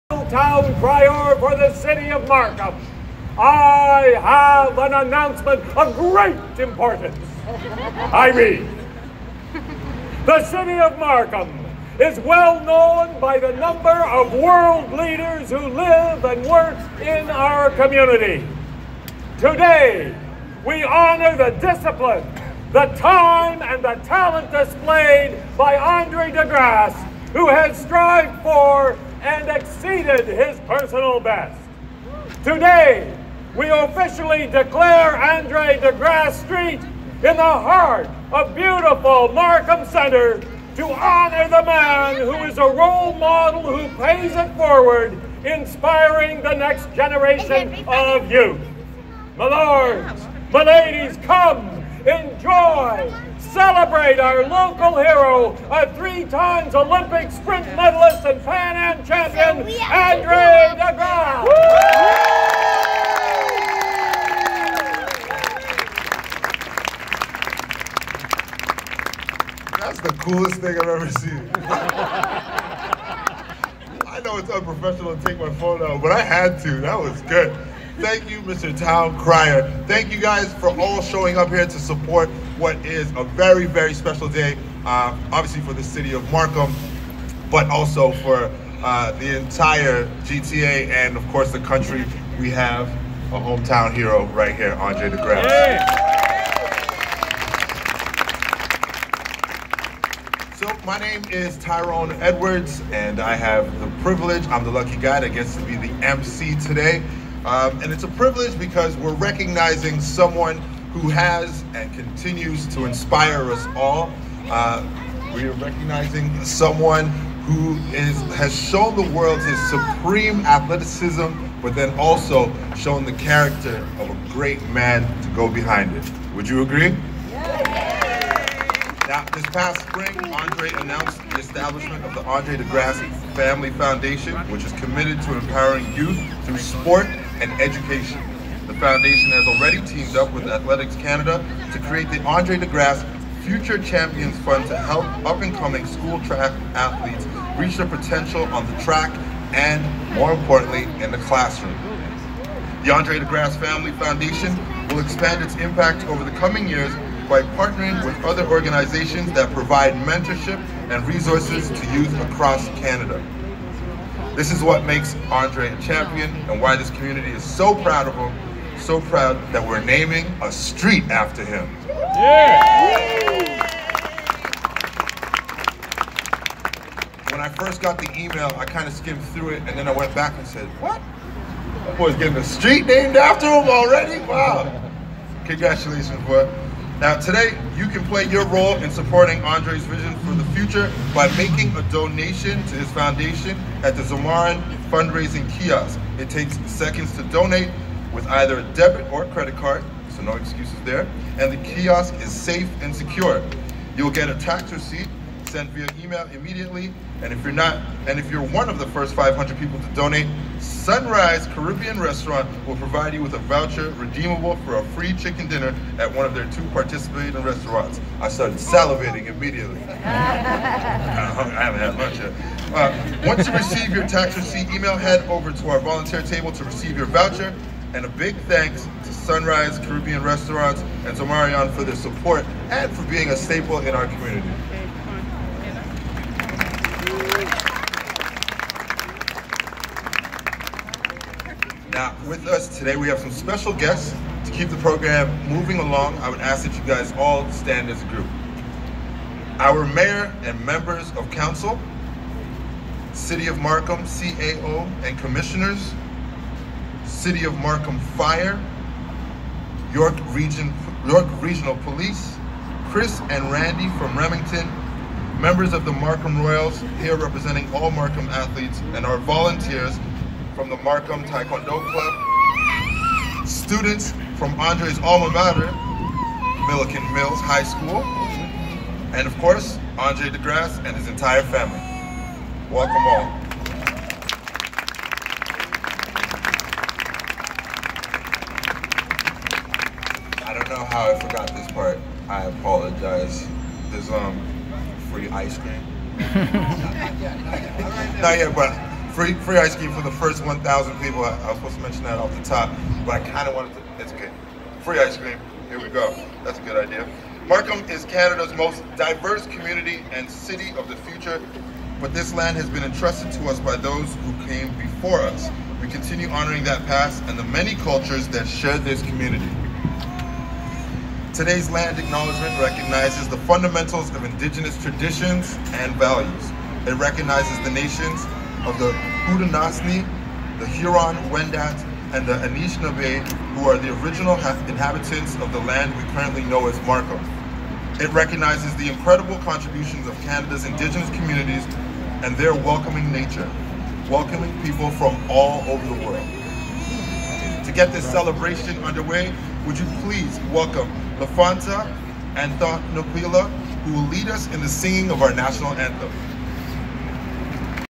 Town Crier, John Webster, Andre De Grasse, Markham, Ontario, Canada
John Webster -Town Crier for the City of Markham, doing his duties at the Andre De Grasse Street Dedication, Markham, Ontario, Canada on September 22, 2018